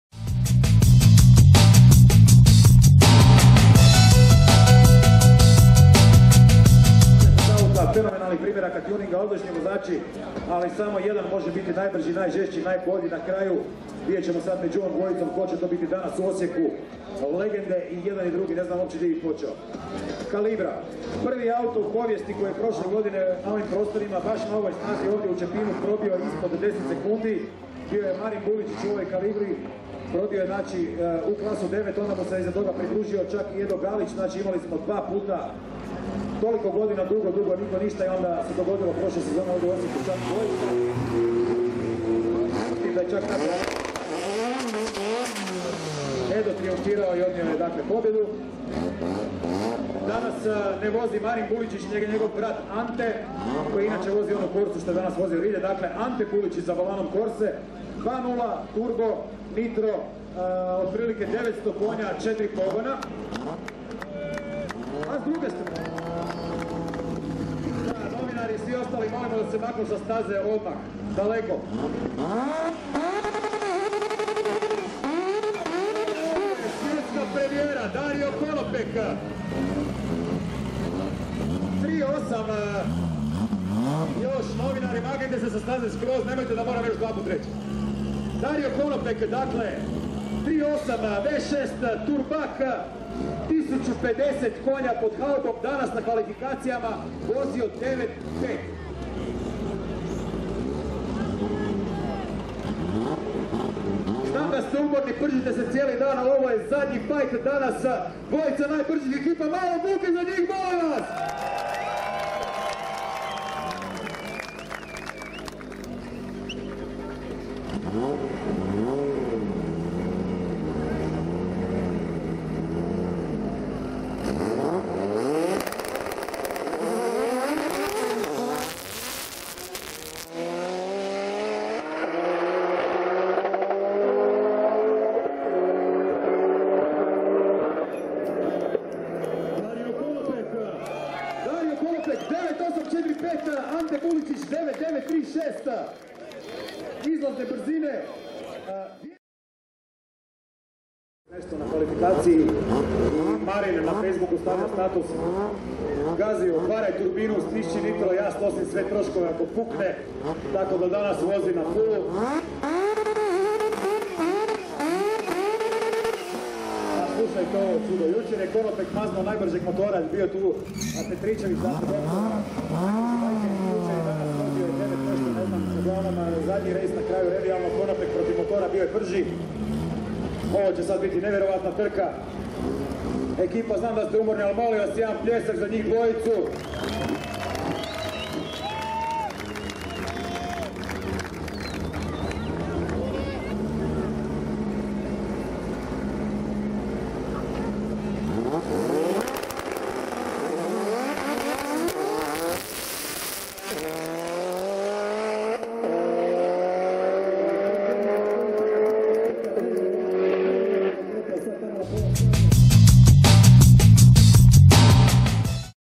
2014 Osijek Street Race Show 12 - Borba Divova
Prvi put u istoriji trka ubrzanja 1na1 je bilo vise vozila u najbrzoj klasi 9 u odnosu na sporiju.  Ovo je borba divova, sve bivsi rekorderi.